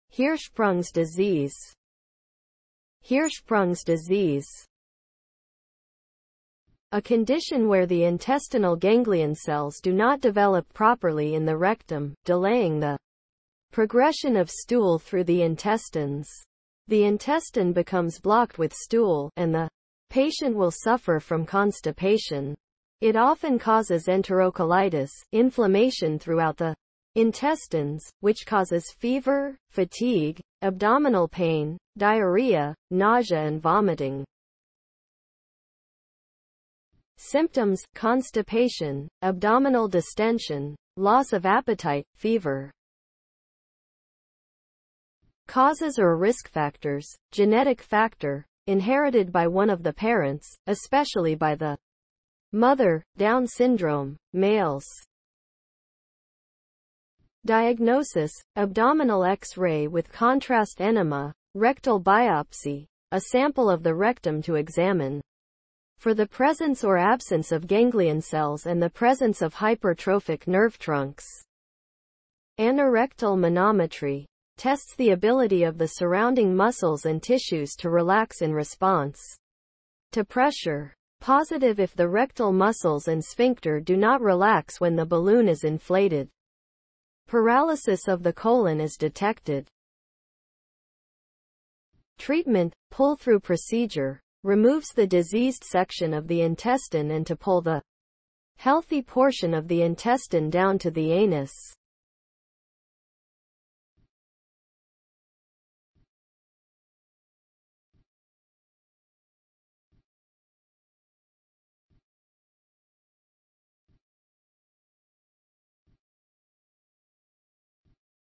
Hirschsprung's disease
(congenital aganglionic megacolon)

A condition where the intestinal ganglion cells do not develop properly in the rectum, delaying the progression of stool through the intestines.
The intestine becomes blocked with stool, and the patient will suffer from constipation.
It often causes enterocolitis (inflammation throughout the intestines), which causes fever, fatigue, abdominal pain, diarrhea, nausea and vomiting.

(Symptoms)
• constipation
• abdominal distention
• loss of appetite
• fever

(Causes or risk factors)
• genetic factor: Inherited by one of the parents (especially by the mother)
• Down syndrome
• males

(Diagnosis)
• abdominal x-ray with contrast enema
• rectal biopsy: A sample of the rectum to examine for the presence or absence of ganglion cells and the presence of hypertrophic nerve trunks.
• anorectal manometry: Tests the ability of the surrounding muscles and tissues to relax in response to pressure. Positive if the rectal muscles and sphincter do not relax when the balloon is inflated, paralysis of the colon is detected.

(Treatment)
• pull-through procedure: Removes the diseased section of the intestine and to pull the healthy portion of the intestine down to the anus.